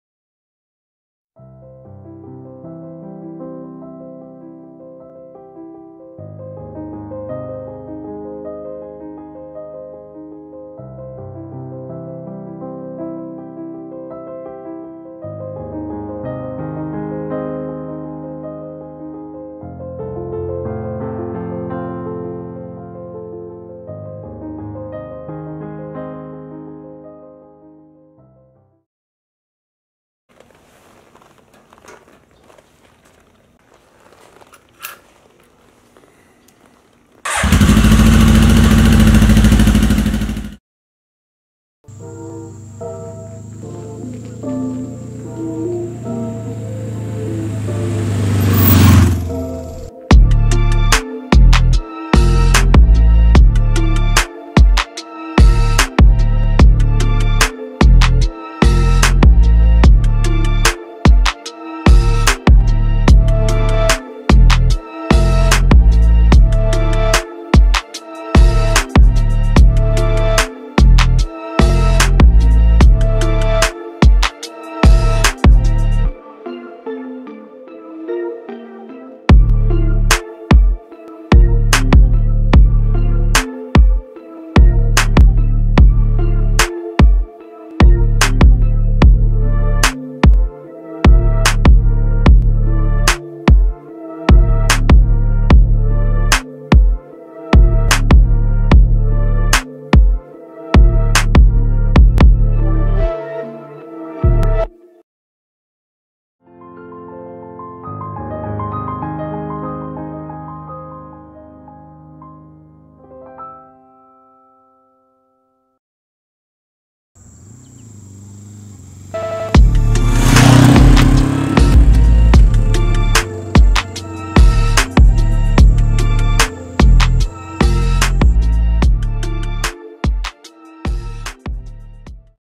Daydream - Honda CB900
Instead of doing what I usually do and only taking still photos, I put together a short video as a personal project just for fun.
Bike is a 1980 Honda CB900 Custom.
Shot on Canon 80D with 24-70mm.